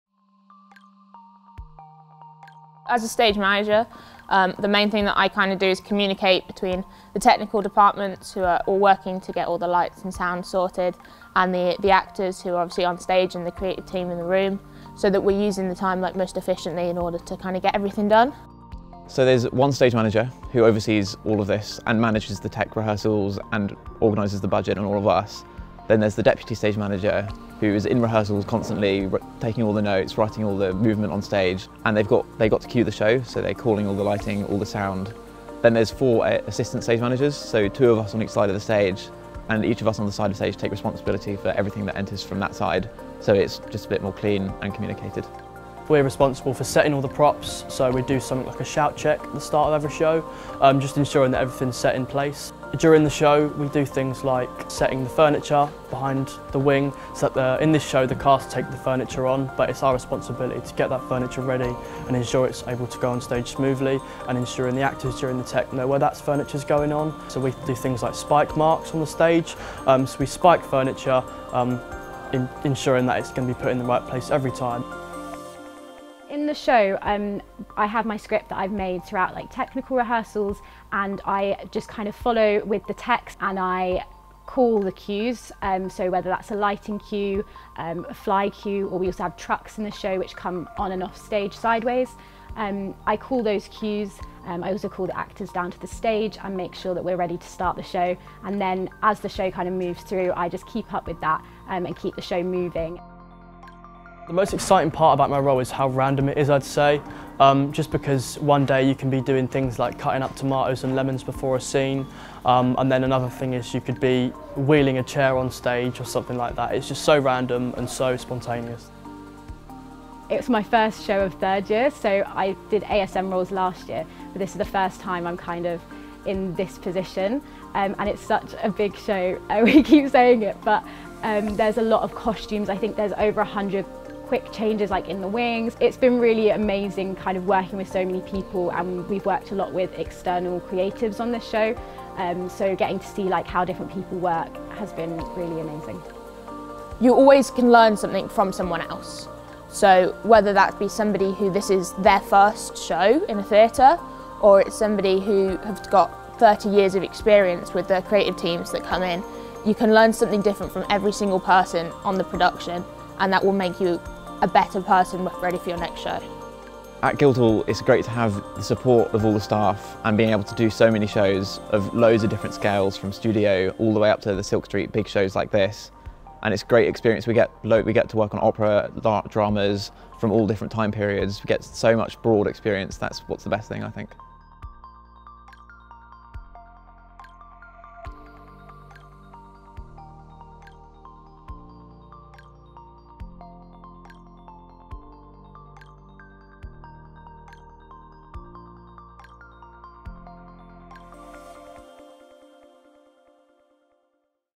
Stage Managing a Guildhall Drama Production
"You always can learn something from someone else. So whether that be somebody who this is their first show in a theatre or it's somebody who has got 30 years of experience, with the creative teams that come in, you can learn something different from every single person on the production and that will make you a better person ready for your next show."

Stage Manager Kate Buxton, Deputy Stage Manager Emma Peace and Assistant Stage Managers Tom Licence & Beri Valentine share what it's like to work in the Stage Management team on a Guildhall production, what their roles involve and what they enjoy about working on productions at Guildhall.

Find out more about this production of Anna Karenina, which is available to watch free and on demand from 9-16 December 2021

Anna Karenina
by Leo Tolstoy
Adapted by Marina Carr

Derek Bond - director
Amy Jane Cook - designer
Jamie Platt - lighting designer
Dyfan Jones - composer/sound designer
Lucy Cullingford - movement director
Dan Light - video designer
Yarit Dor - fight & intimacy director
Jonathan Chan - assistant lighting designer
Emmy Briggs - assistant director

Cast

Anna Karenina – Annie Cordoni
Alexei Karenin – Tom Coleman
Grisha/Seryozha – Connor Porter

Princess Sherbatsky – Lois Pearson
Prince Sherbatsky – Jonny Burman

Dolly – Ema Cavolli
Stiva Oblonsky – Samuel Armfield
Tanya/Women 2 – Sarah Slimani
Masha/Women 1 – Amma-Afi Osei
Doctor/Ambassador/Vanya – Aaron Gill
Nikolai Oblonsky – Séamus McLean Ross

Kitty – Anabella Jennings
Kostya Levin – Kishore Walker

Countess Vronskaya – Boni Adeliyi
Alexei Vronsky – Matthew Broome
Countess Nordston – Julé Michelo
Princess Myakhaya – Maisie Ayres
Tushkevich – Tyreke Leslie
Countess Lydia – Sesley Hope
Dance Master/Turovtsyn – Diyar Bozkurt

Betsy/Dance Captain – Laura Euler-Rolle
Betsy’s Husband – Jake Dunn
Ambassador’s Wife/Assistant Dance Captain – Frankie Hart

Matvei – Tyler-Jo Richardson
Ignat – Os Leanse
Nanny – Isobel Fairchild

Production Team

Construction Manager
Charlotte Cross

Scenic Carpenters
Mary Deakes
India Mills

Assistant Carpenters
Issy Jordan
Finley Wellspring

Costume Supervisor
Amy Hill

Wardrobe Manager
Olivia Hilton-Foster

Costume Assistants
Jack Garner-Greene
Sophie Presswell

Production Electrician
Adi Currie

Lighting Programmers
Bradley Halliwell
Ethan Howlett

Production Manager
Emma Horne

Technical Manager
Rosie Roberts

Production Assistants
Stanley Olden
Alex Gasson-Gray

TA/Automation Operator
Jonny Reeks

Head of Flys
Georgie Bottone

Prop Makers
Takiyah Campbell
Kyra Coppini
Ema Cunha

Scenic Art Co-ordinator
Chloe Rushbrook

Scenic Artist
Allegra Totaro-Wainwright

Assistant Scenic Artists
Holly Hooper
Fran Johnson
Levi Meehan-Powell

Video Engineer & Operator
Ben Shepherd

Production Sound Engineer
Sam Ferstenberg

Sound Operators & Mic Runners
Ros Chase
Ashe De Waal
Ben Mills

Sound no.1 (mixer)
Anja Urban

Stage Manager
Kate Buxton

Deputy Stage Manager
Emma Peace

ASM/Book Cover
Sammy Lacey

Assistant Stage Managers
Iben Bering Sørensen
Tom Licence
Beri Valentine

Additional Production Staff
Freelance Staff Stage Management Supervisor
Laura Flowers

The School would like to thank Marcus Hall Props and the Almeida Theatre for lending props for this production.